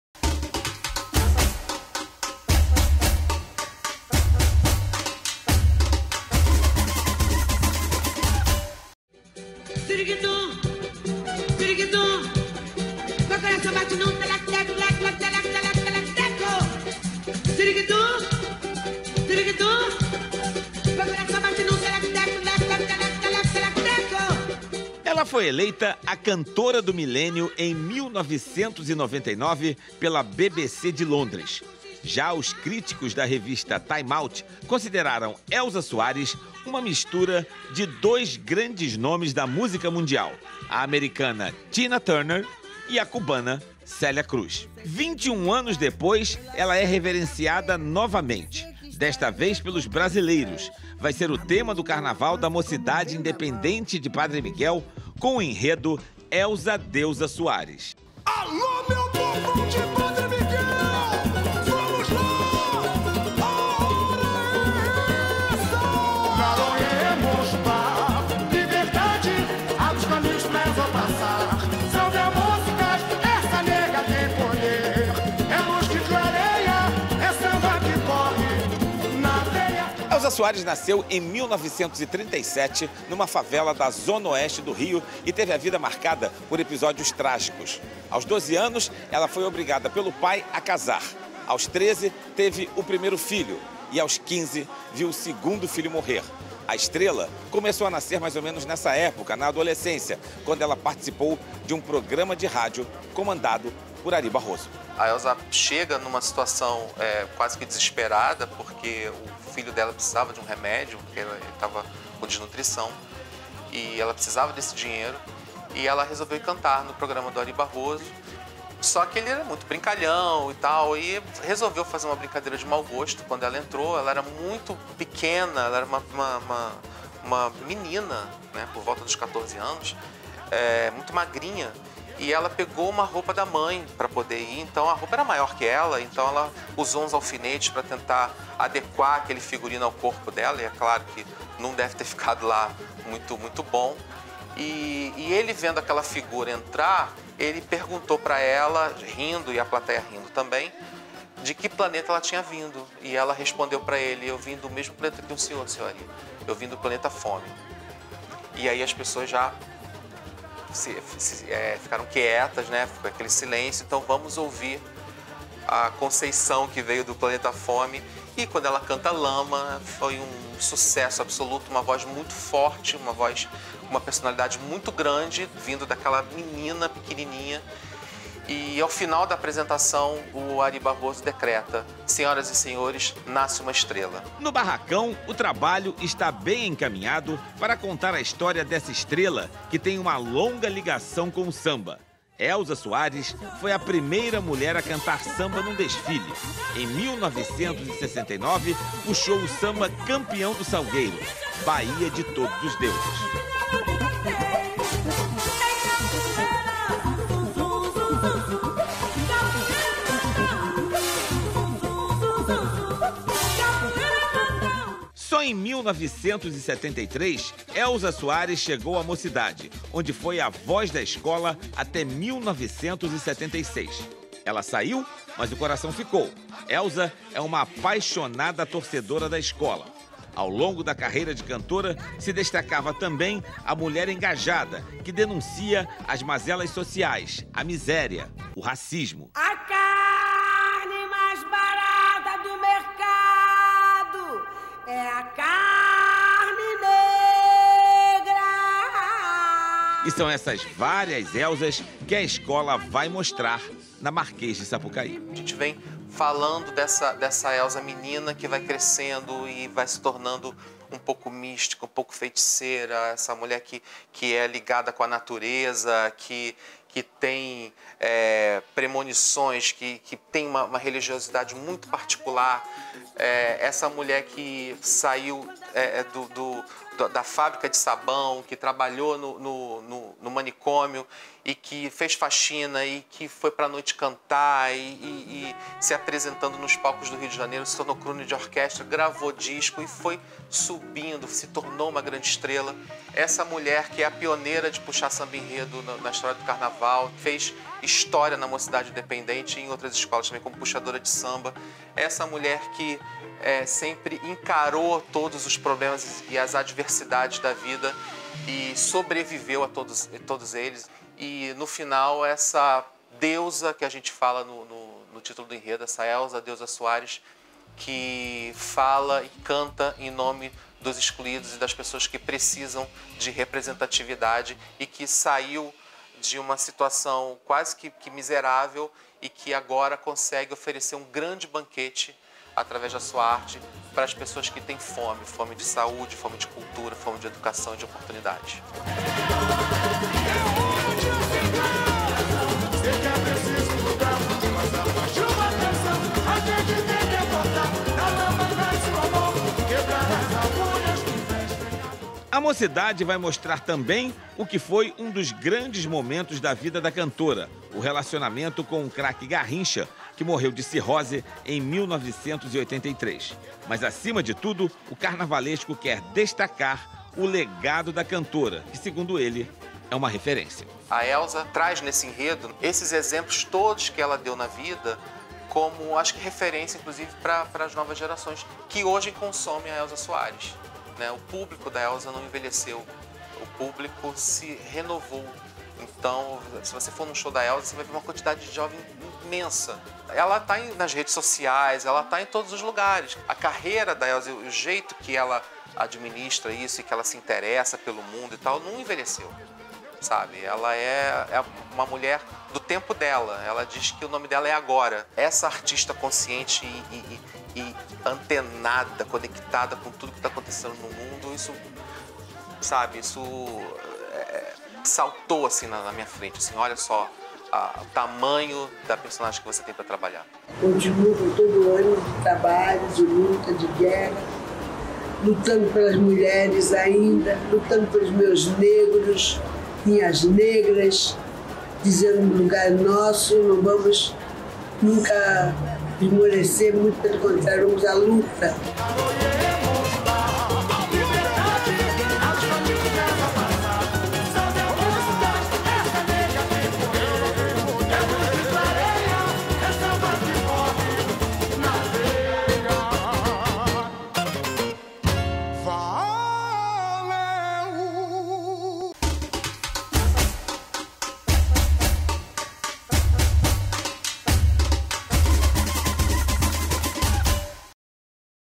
Carnaval 2020 - Mocidade Independente de Padre Miguel - 29.01.2020
A série Carnaval 2020 vai mostrar os preparativos das escolas de samba do grupo especial para o desfile no Sambódromo. Os sambas-enredo, o trabalho nos barracões, os principais personagens e os enredos que prometem levantar a avenida.